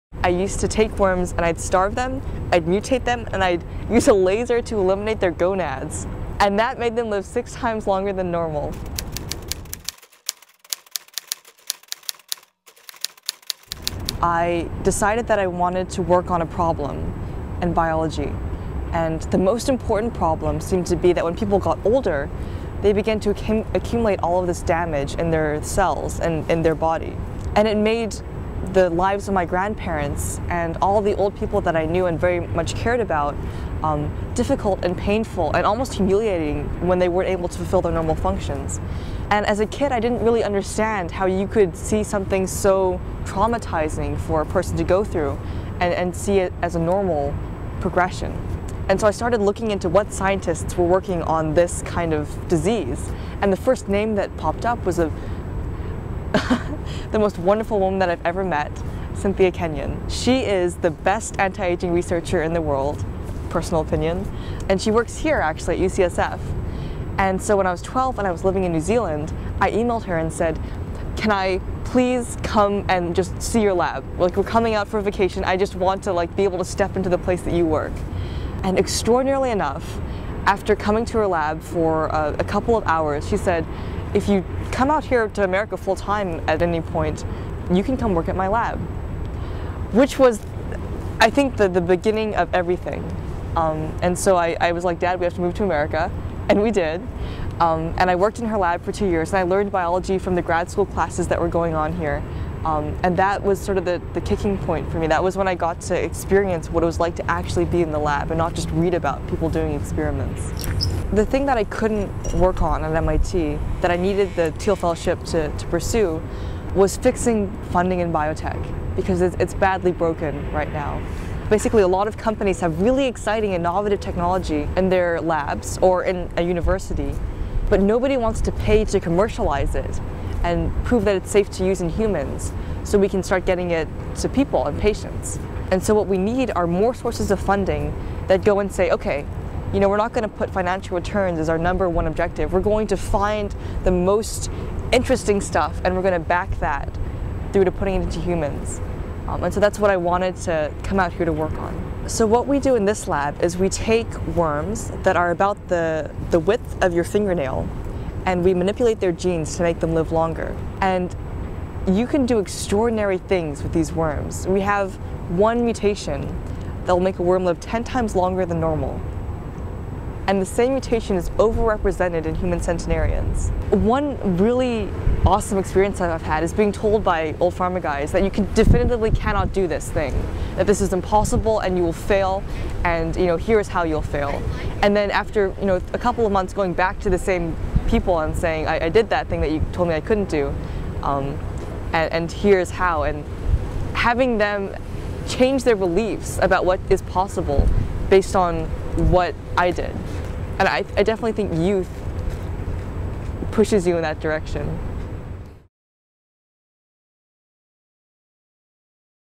PandoList: 18-Year-Old Laura Deming Vows To Slow Down Aging Process
Laura Deming dropped out of college to take advantage of the Thiel Fellowship so that she could fix funding in biotech.